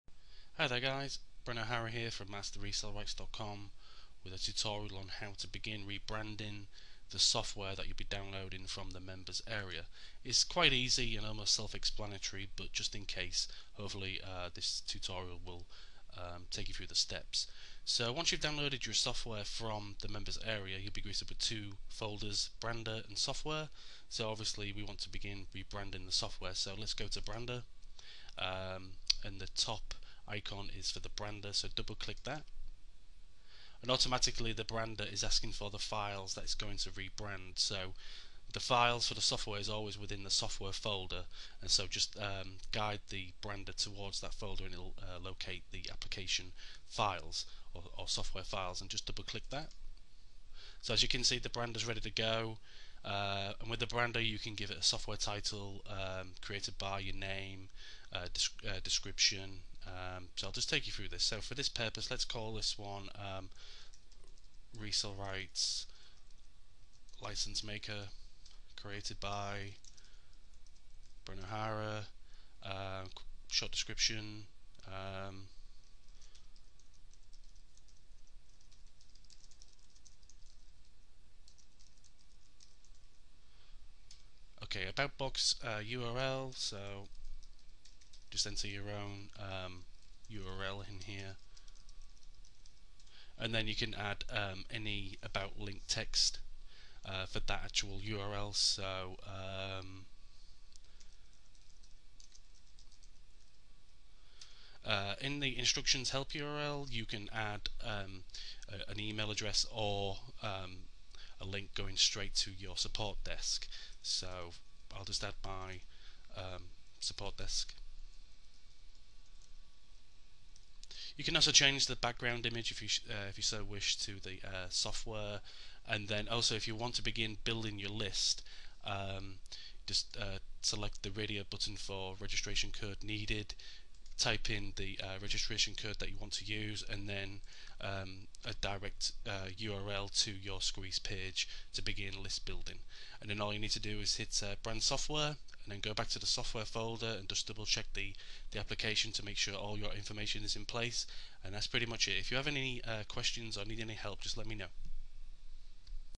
Rebranding Software From MasterResellRights.com
A quick tutorial on how to begin rebranding the software